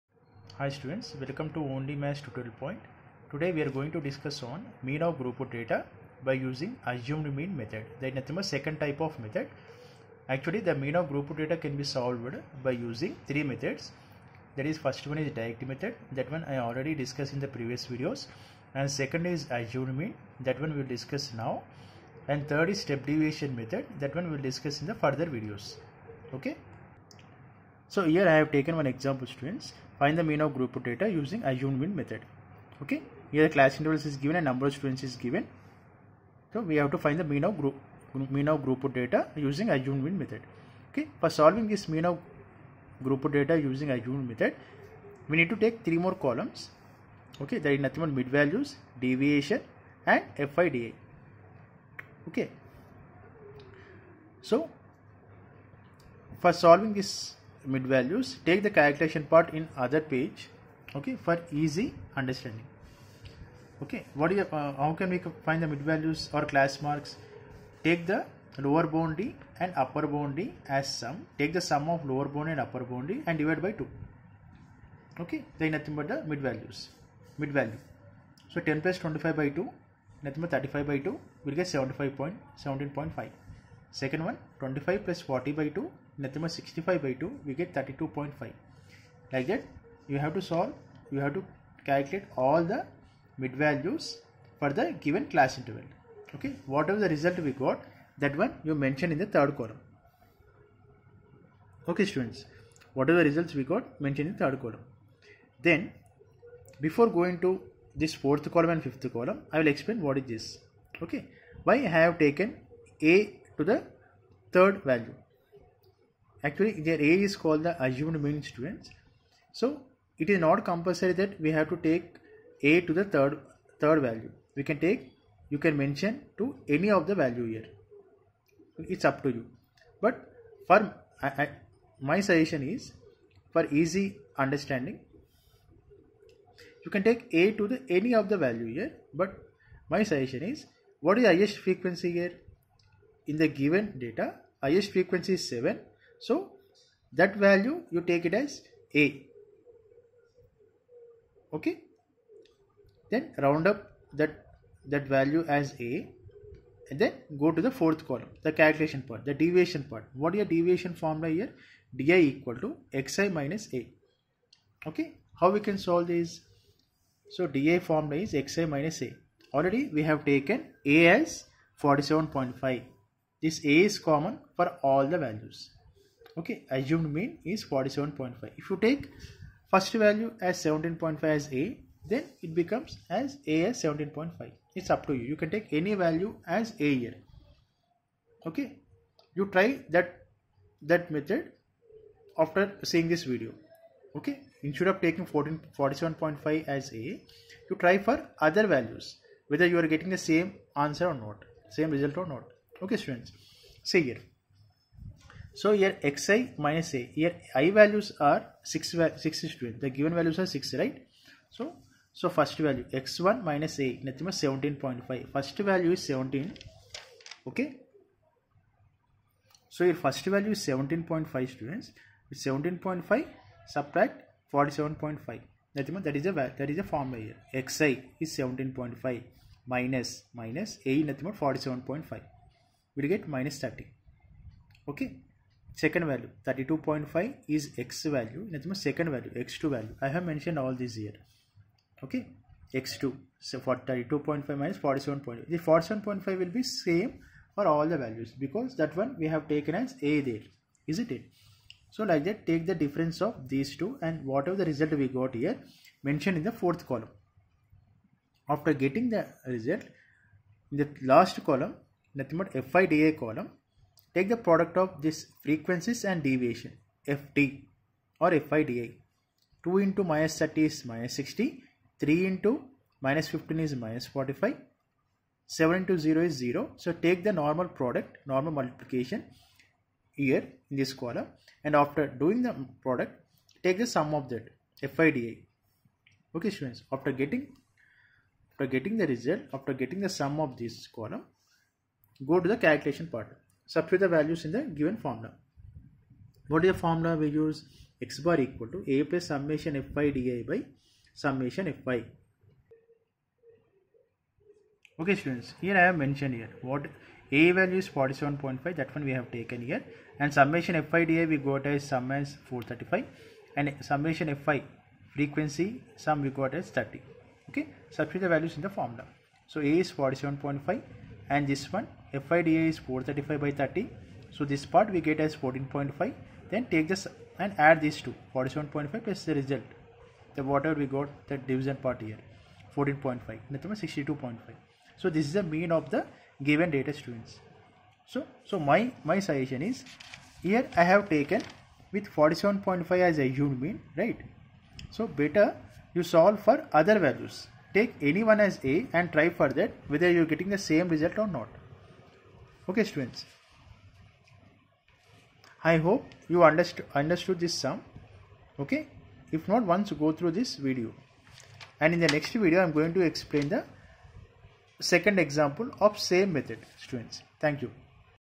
Statistics-Class-X Mean of grouped data using Assumed Mean method
Mean of grouped data using Assumed mean method.Explained with an example .